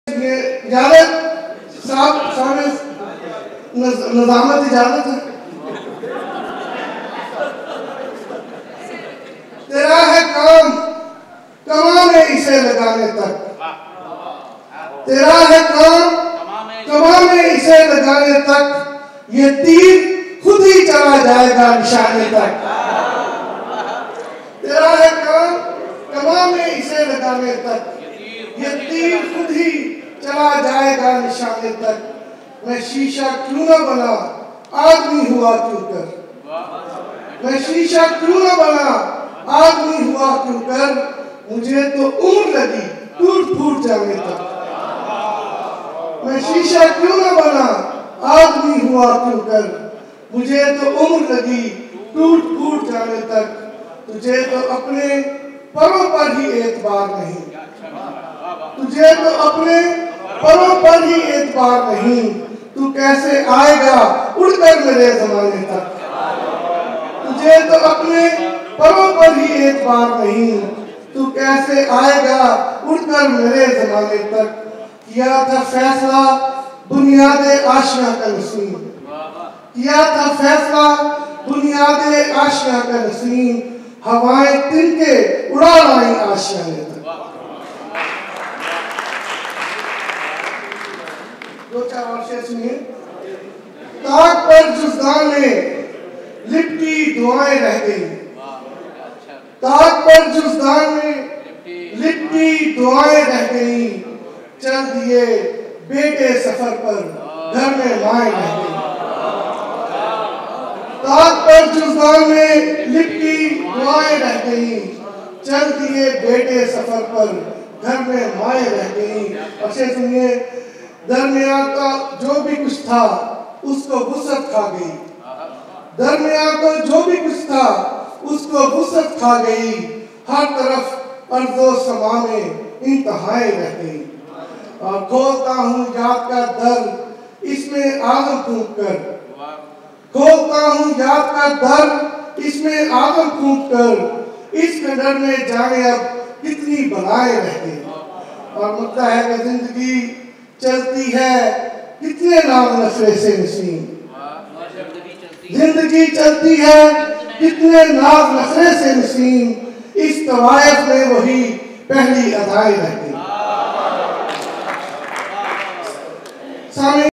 Ifti Nasim - Ghazal - Tera hai kam kuman mein ise lagany tak
Legendary Ifti Nasim reciting his poetry during a Sydney Recital of 2007